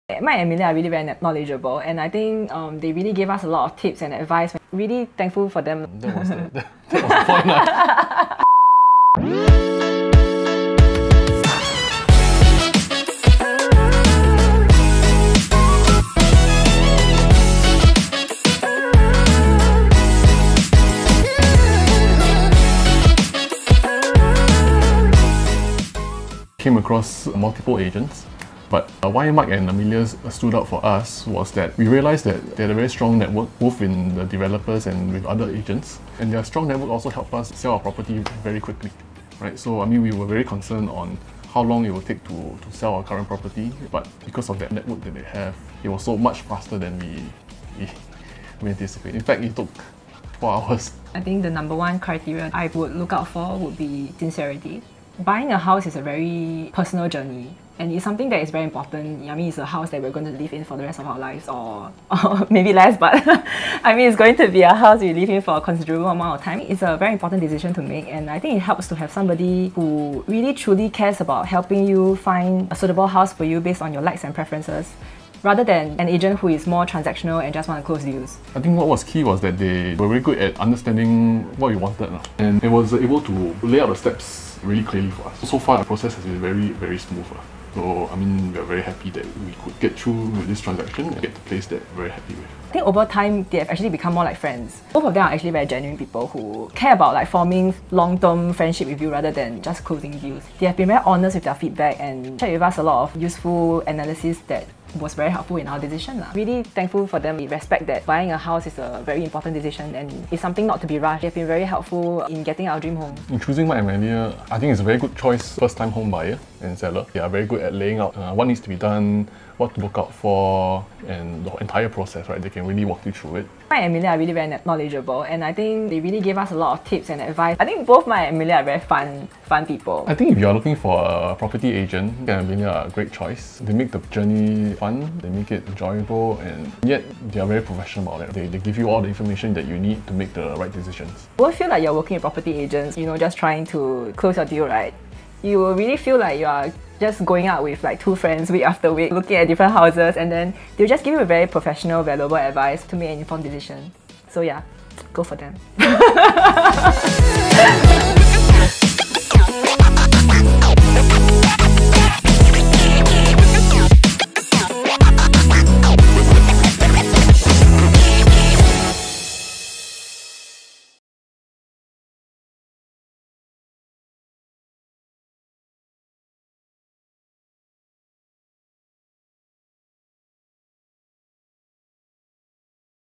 From Clients to Friends, Selling Home in 4 Hours? | Real Talk EP1
In our FIRST Episode of REAL TALK by People of Property, follow Mike and Amelia as they interview Nic and Kay on their property journey in selling their home. How was their entire experience and what do they think about Mike and Amelia.

Chapters:
0:00 - Introduction & Opening
0:26 - Why did you choose to engage Mike & Amelia
0:51 - What is important to you in choosing a property agent?
1:18 - How has it been working with Mike and Amelia?
2:41 - Contact Details
FOR VIEWING REQUEST PLEASE CONTACT

TO LIST YOUR PROPERTY PLEASE CONTACT